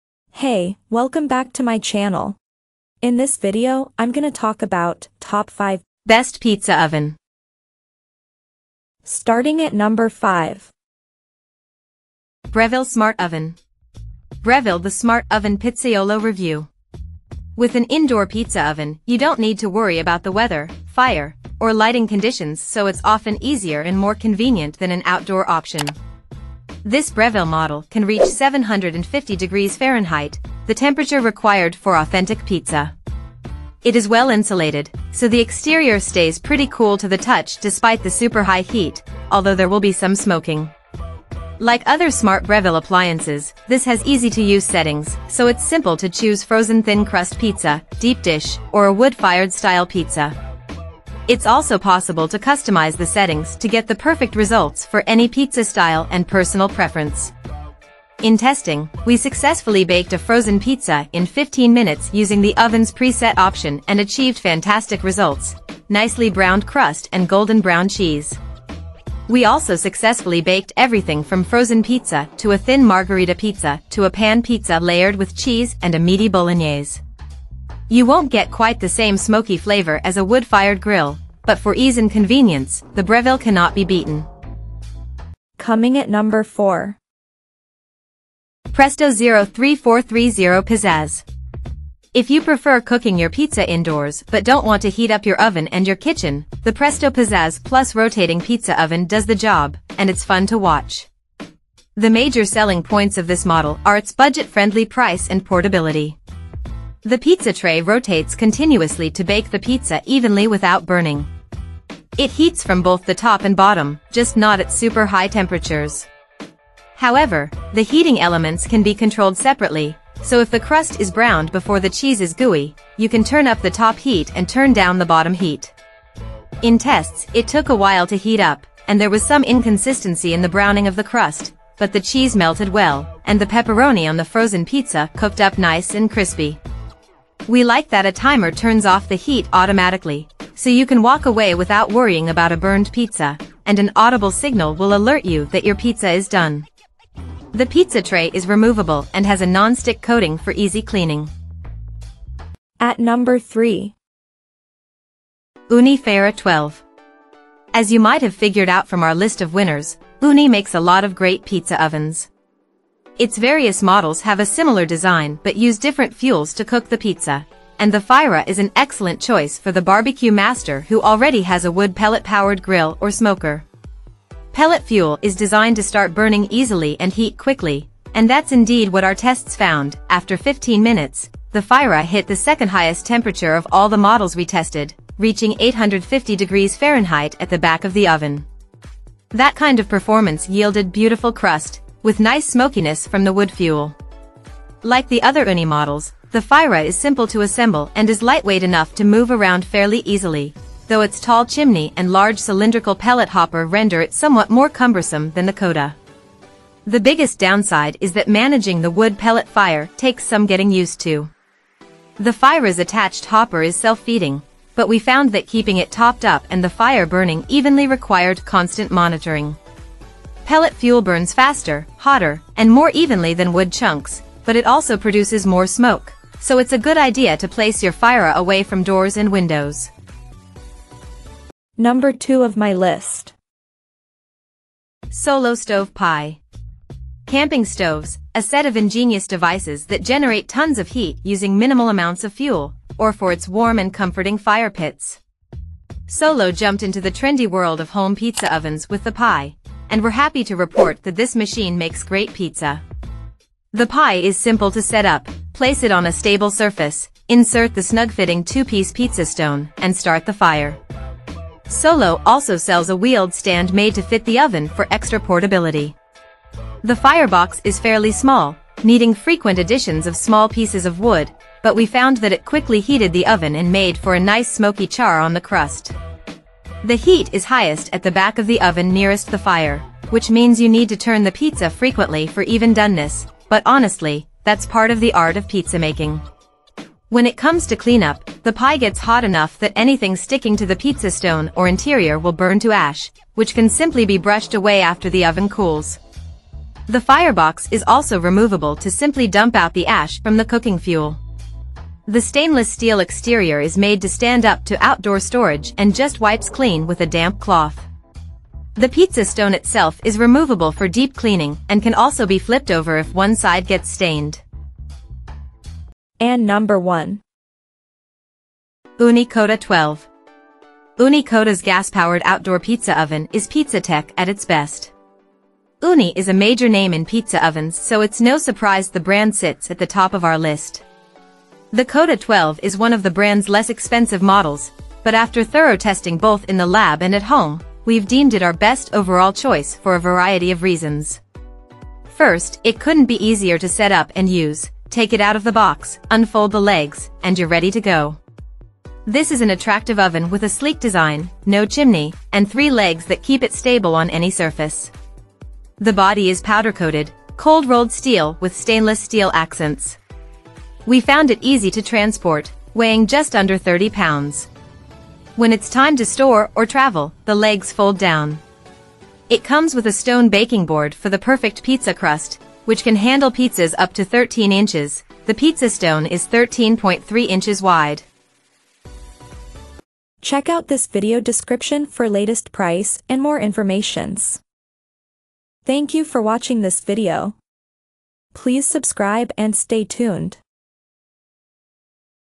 Top 5 Best Pizza Oven in 2024
Here is the Updated Prices of the Best Pizza Oven  in 2023!

Things that we mentioned in this video:
1. Ooni Koda 12  - B07PN5R5WY
2. Solo Stove Pi  - B0B7XXXCZG
3. Ooni Fyra 12 - B088385N9Z
4. Presto 03430 Pizzazz  - B00005IBXJ
5. Breville Smart Oven - B08R8YZR32